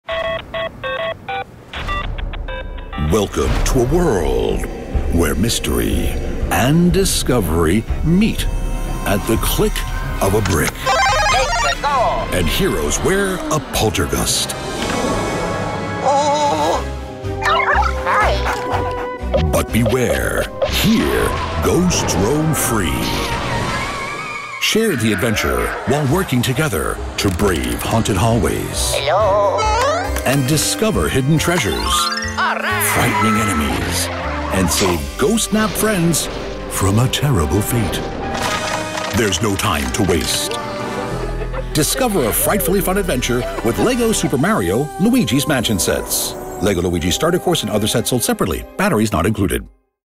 LEGO® Super Mario™ - Luigi's Mansion™ Expansion Set: Ready for a frightfully fun adventure?
Turn family night into family fright night with LEGO® Super Mario™ Luigi’s Mansion™ Sets — out now! Defeat ghosts and discover hidden treasures in the hallways of the mysterious mansion. Dim the lights, cue the eerie music and start your adventure today!